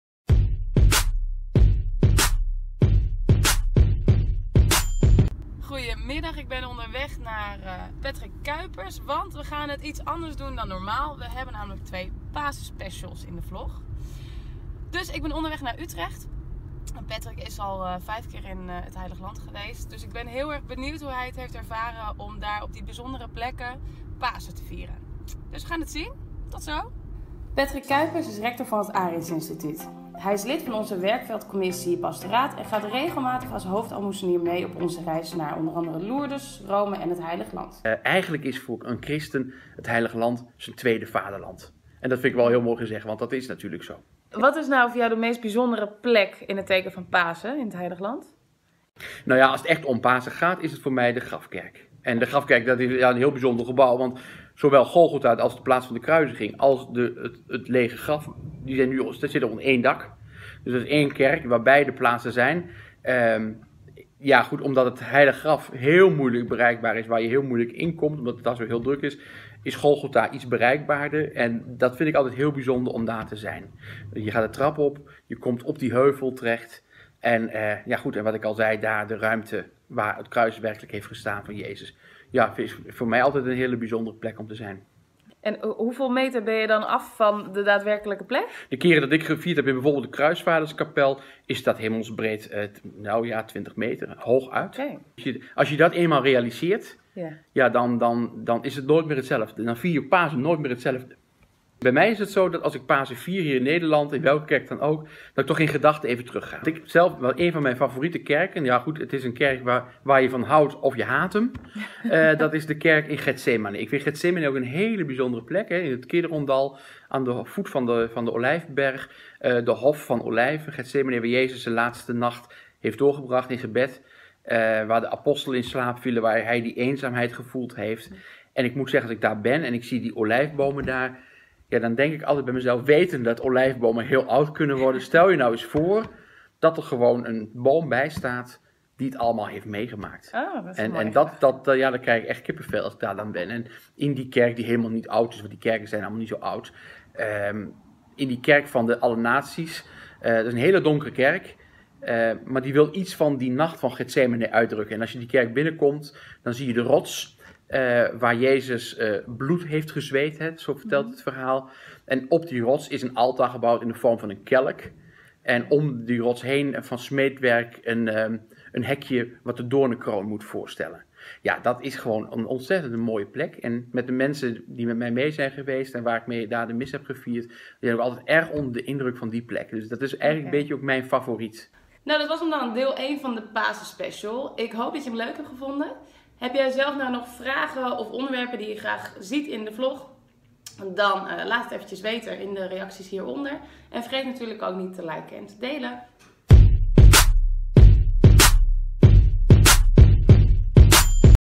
Vlog #3 Paasspecial deel 1 Vieren in het Heilig Land
Emke de Bruijn, Manager Touroperating bij VNB, neemt jou in haar vlog mee in de voorbereidingen van de Nederlandse Dagen in 2018 in het Heilig Land. In deze vlog, deel 1 van twee speciale Pasenspecials, heeft Emke een gesprek met rector Patrick Kuipers over Pasen en vieren in het Heilig Land.